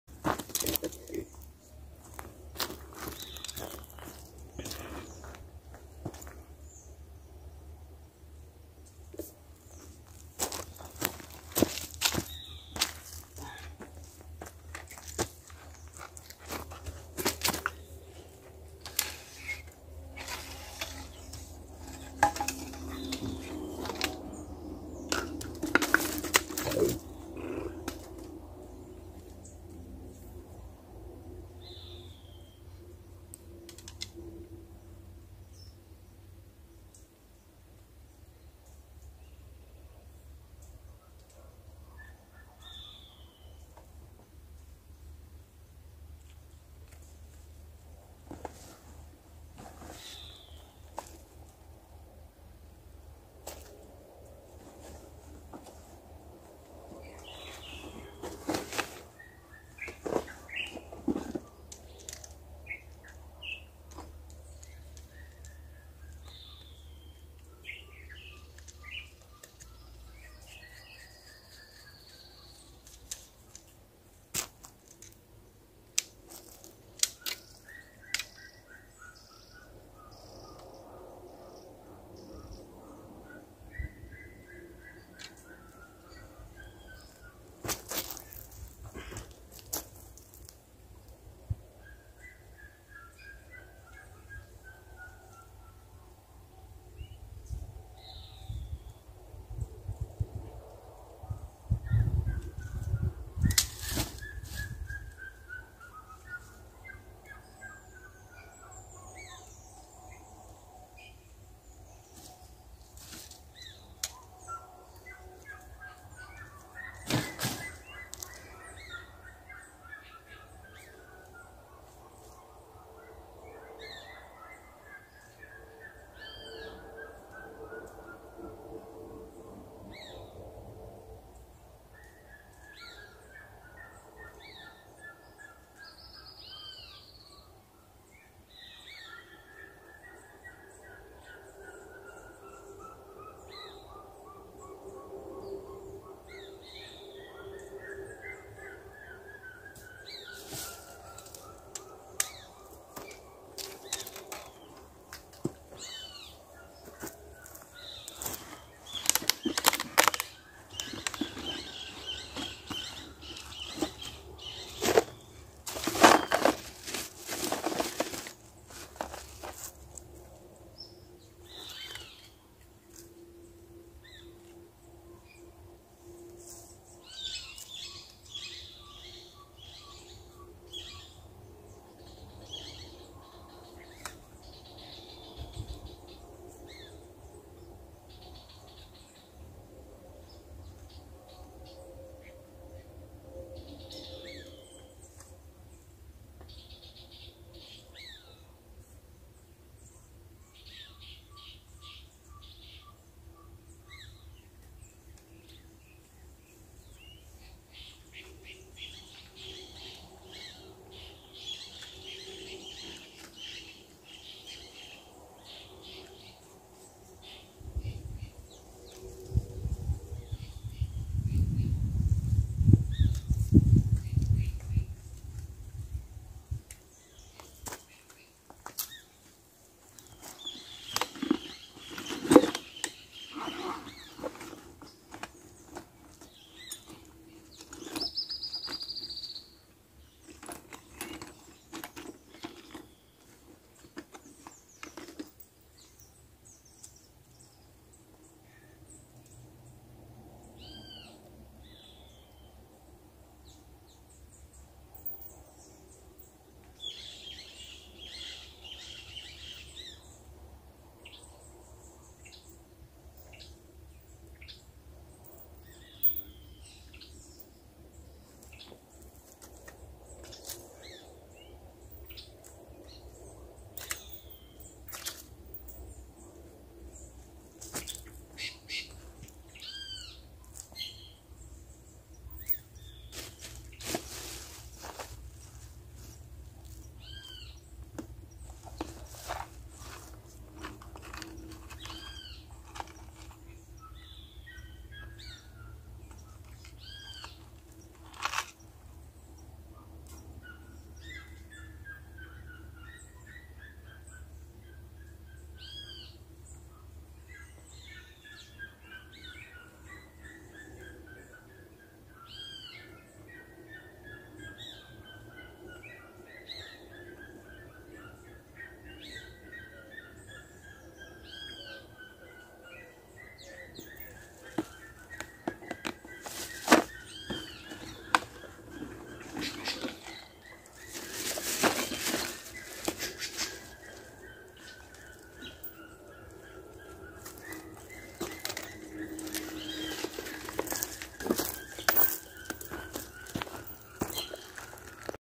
arrumando as traia para começar a pesca
nesse vídeo eu preparo os equipamentos pra a pesca ! acompanha ae vai ser bom demais!!